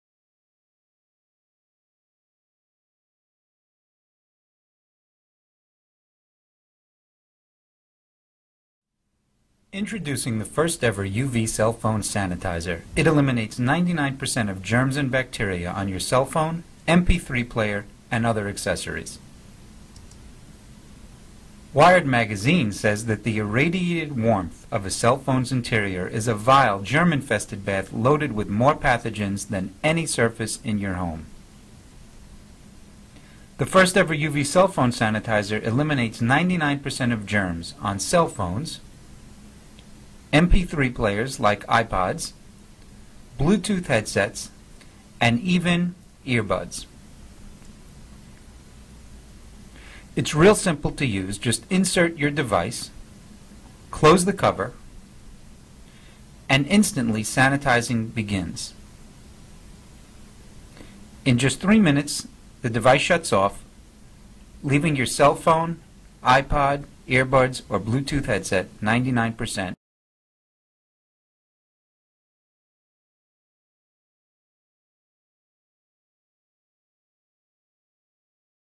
Cell Phone Sanitizer
This cell phone sanitizer is the first of its kind, allowing you to destroy 99.9% of surface bacteria, while keeping your technology safe. Proven germicidal UV light is the power behind the sanitation.